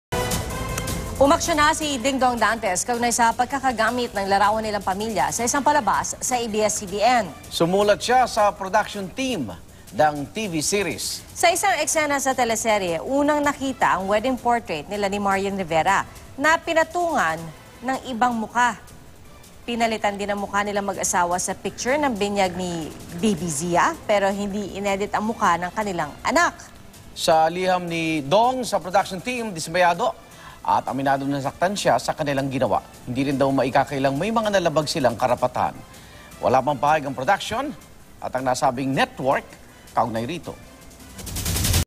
Saksi: Dingdong Dantes, sumulat sa production team ng isang TV series ng ABS-CBN
Umaksiyon na si Dingdong Dantes kaugnay ng pagkakagamit ng larawan nilang pamilya sa isang palabas sa ABS-CBN.